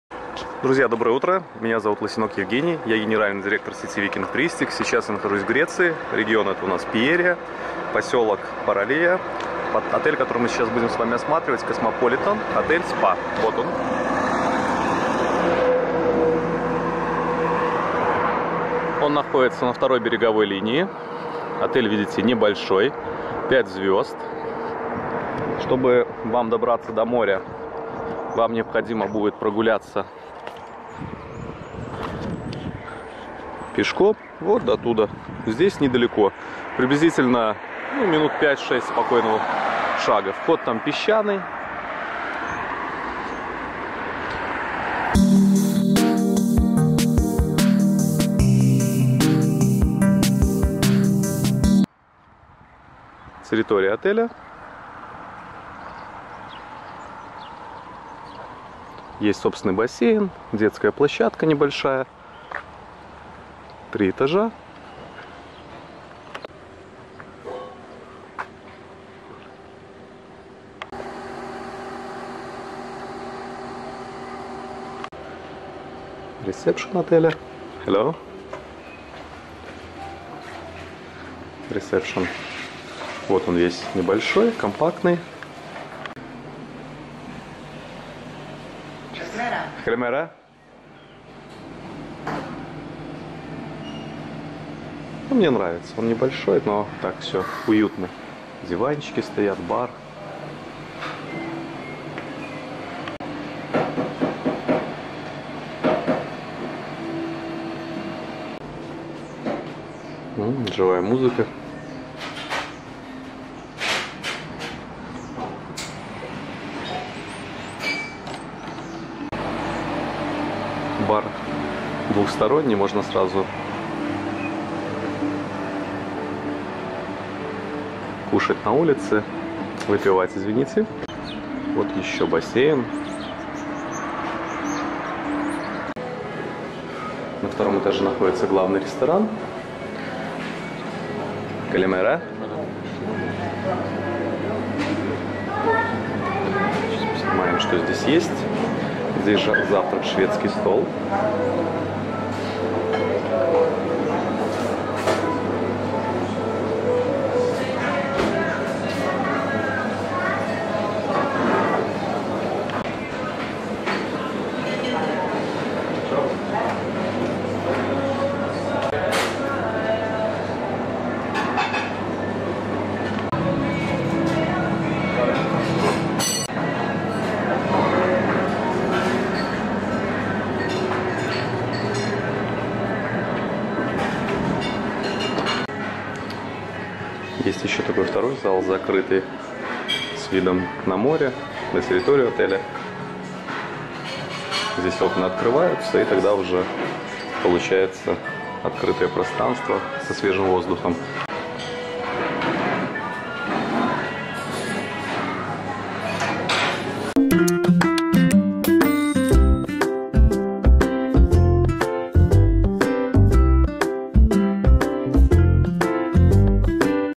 Обзор отеля Cosmopolitan Hotel 4* (Космополитан Хотел), Греция, Паралия Катерини (Пиерия). 2018
Обзор отеля Cosmopolitan Hotel & Spa 4* (Космополитан Хотел Спа), Греция,  Паралия Катерини (Пиерия). 2018

Cosmopolitan Hotel & SPA - это современный оригинальный отель, расположенный недалеко от города Катерини , в тени горы Олимп, и предлагающий своим гостям роскошь, комфорт и незабываемый отдых. Здание отеля имеет оригинальный дизайн, делающий его похожим на корабль или шикарную белоснежную яхту, неторопливо покачивающуюся на волнах. Здесь вас ожидает теплая гостеприимная атмосфера и высокий уровень обслуживания.  Подходит для романтического и семейного отдыха.

Мы, турагенство "Викинг Туристик" , находимся:
г.Минск, ул. Румянцева 17, офис 2

                 ул. Революционная 17, офис 7.